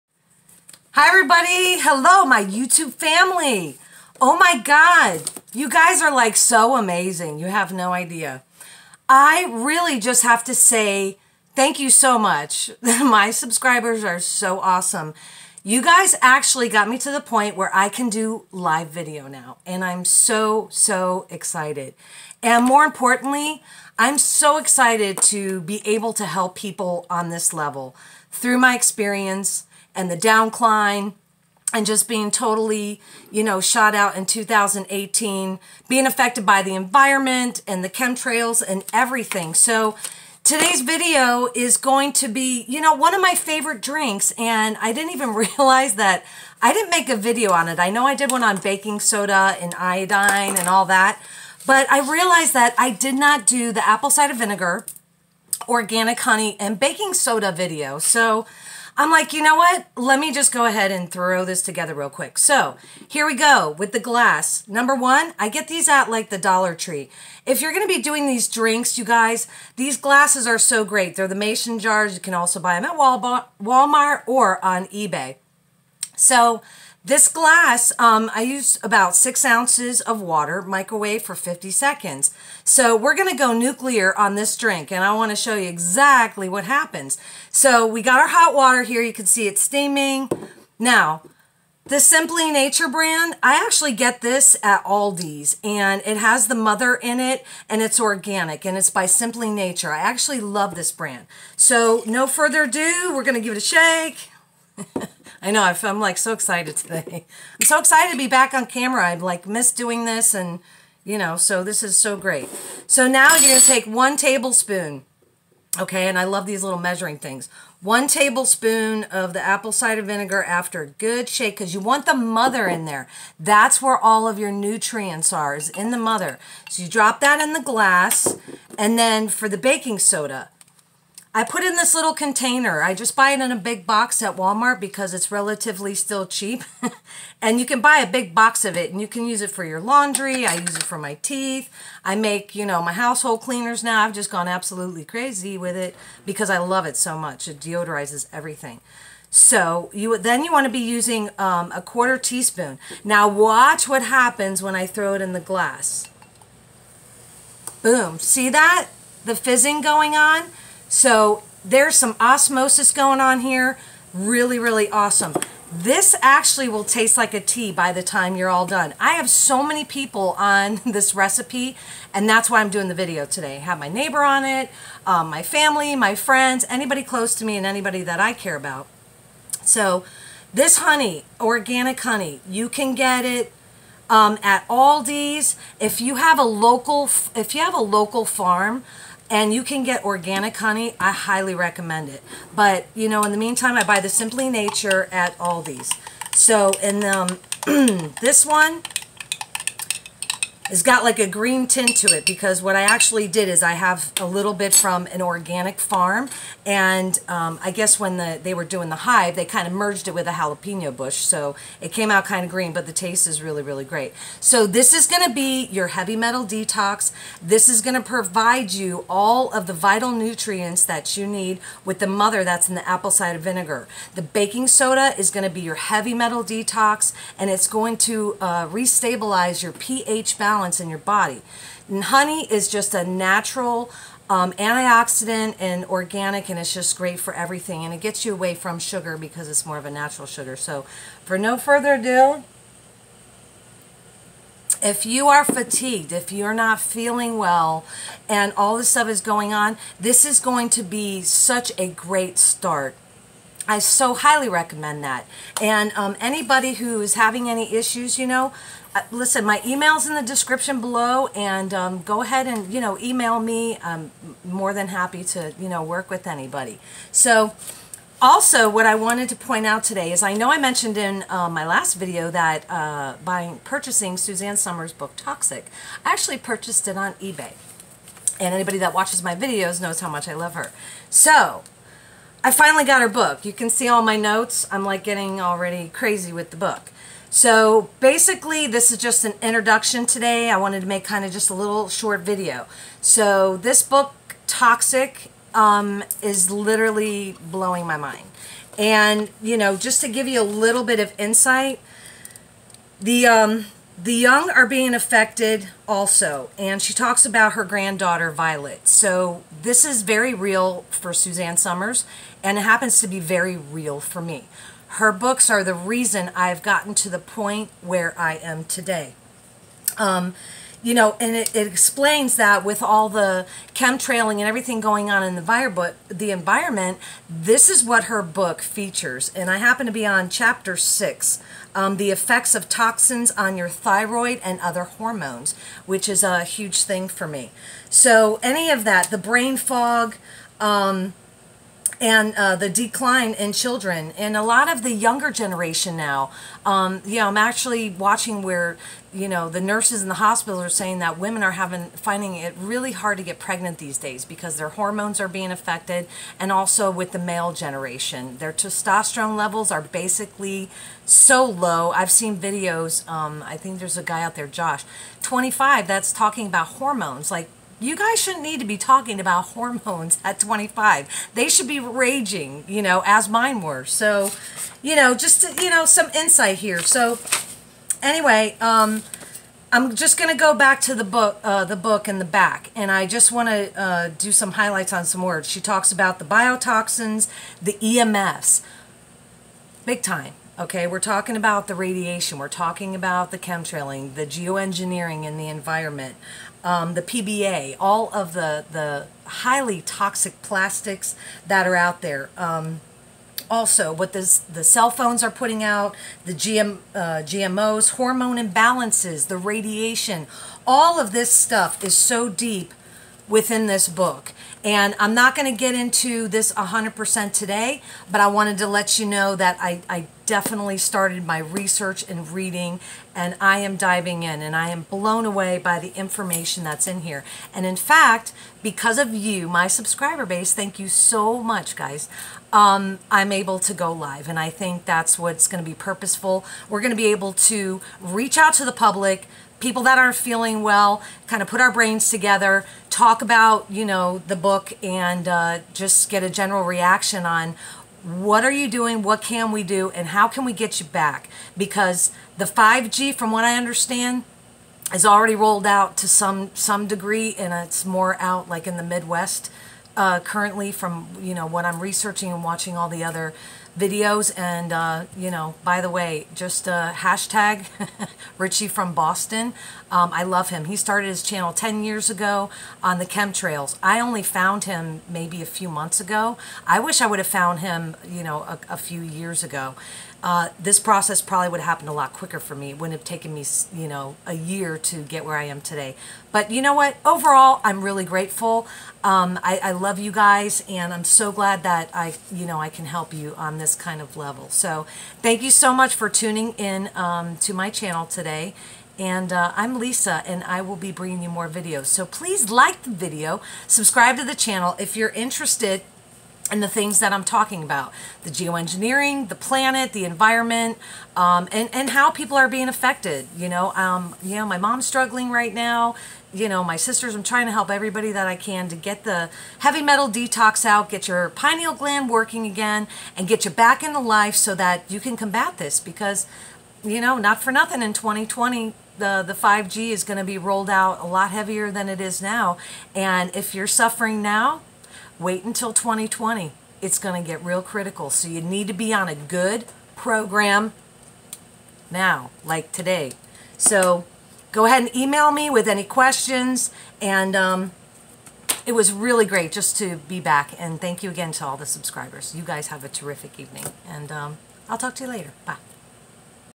HeavyMetalDetox ACV BAKINGSODA & Honey SUZANNE SUMERS TOX-SICK
Published 4.9.2019
Today's video features my most saught after Detox recipe!!
This 💥"Feel Better Solution" has helped me achieve a heavy metal detox while providing relief and giving me back the functionality of my Pineal gland. If you are suffering from brain fog this will help immensely😇
All things mentioned🍀Tox-sick (book)

THANK YOU TO ALL MY SUBSCRIBER'S MAY YA ALL HAVE A BLESSED DAY😎♥